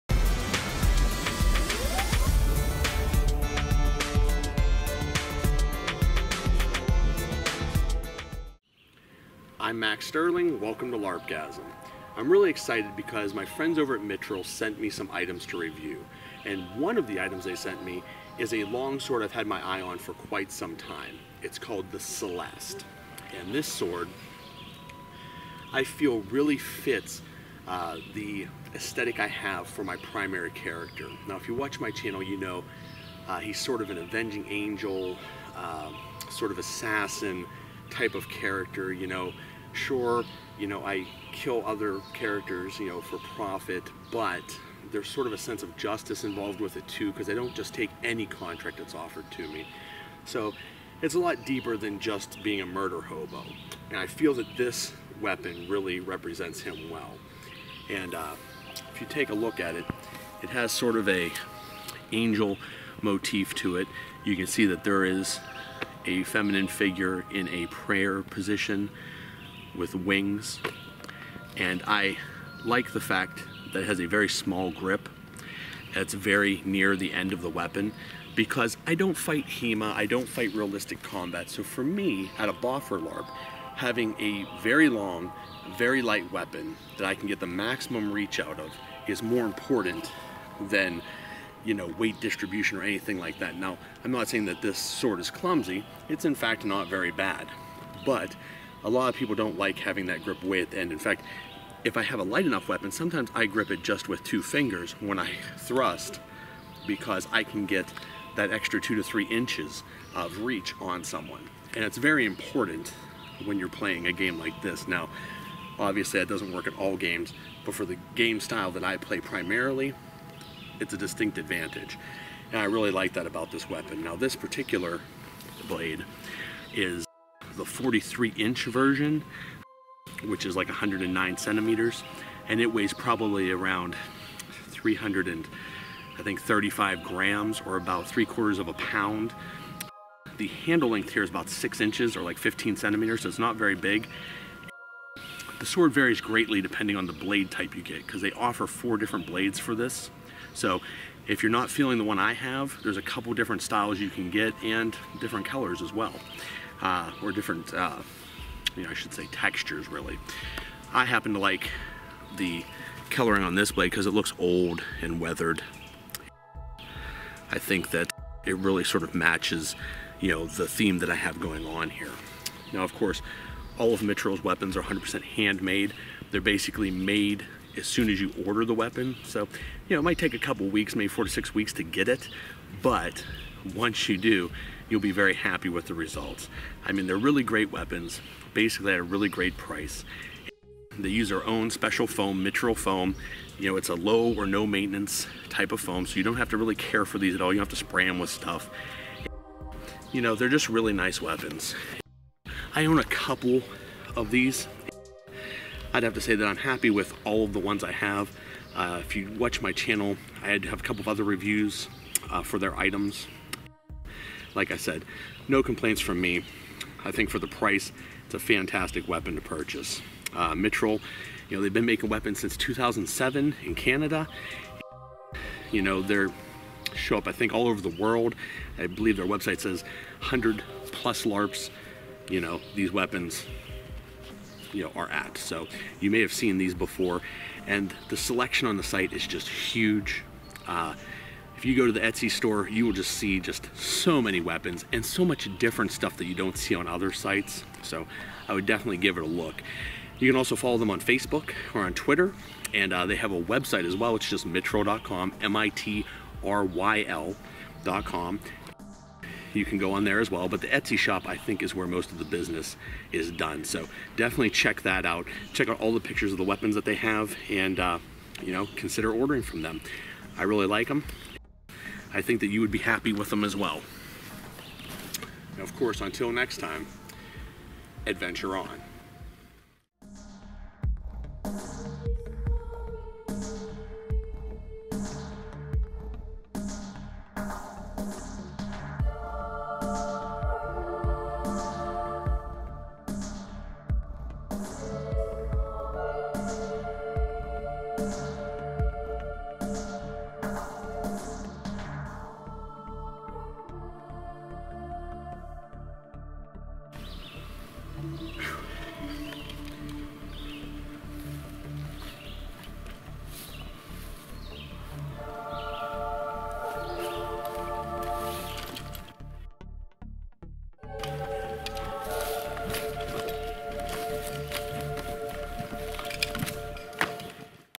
Larp Weapon Review - Celeste Sword by Mitryl
Celeste Sword Courtesy of Mitryl

Maxx reviews the Celeste Sword, which is a foam larp sword by Mitryl.

This is a fantastic sword with great detail and excellent design for a very fair price.  Be sure to check out their store for other awesome designs and customization options.

I personally used this sword at a recent larp and it performed very well.  I had no complaints from hard hits and got many compliments on it's aesthetics.

STATISTICS:

-Length: Varies on Choice
-Blade: Varies on Choice
-Handle Length:6" (15cm)
-Guard width: 7.5" (19cm)
-Grip: Firm
-Weight: Varies on Length
-Core: 3/8" Fiber Glass Rod
-Tip: Kevlar Reinforced
-Foam: Mitryl Foam

If you plan to purchase this most divine weapon, please go

and Follow on Twitter @MitrylCanada

or